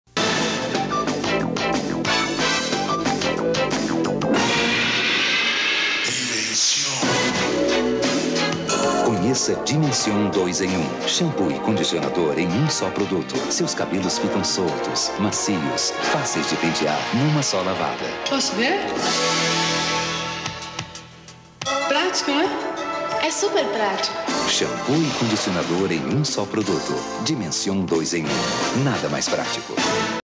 Unilever Brasil - Comercial Lançamento Shampoo Dimension 2 em 1 - 1991
Dimension 2 em 1, anteriormente conhecido com Dimension by Lux (anteriormente apenas Dimension ou New Dimension ) é uma marca de shampoo , fabricada desde 1982 [1] pela divisão de cuidados pessoais da Lever Brothers . Era particularmente distinguível pela garrafa amarela característica . Em 1987 foi o primeiro shampoo a introduzir a fórmula "2 em 1" (shampoo e condicionador ). [2] Em 18 de abril de 1985 , a Lever Brothers reorganizou sua estrutura corporativa [3] e a marca Dimension foi adquirida pela Unilever, passando a ser a divisão de cuidados com os cabelos primeiro da Fabergé e depois da Lux , passando a ser a atual Dimension by Lux, que a comercializa em quatro variantes: para cabelos normais, para cabelos lisos, para cabelos secos e para cabelos oleosos [4] . Entre os depoimentos mais famosos da marca podemos citar a atriz americana Heather Locklear .

Foi lançado oficialmente no Brasil em 1990, com o nome de Dimension 2 em 1. Em 1995, passou por algumas mudanças, e também houve o lançamento da linha Dimension Fruits.
No final dos anos 90 o produto saiu do mercado, retornando em 2007.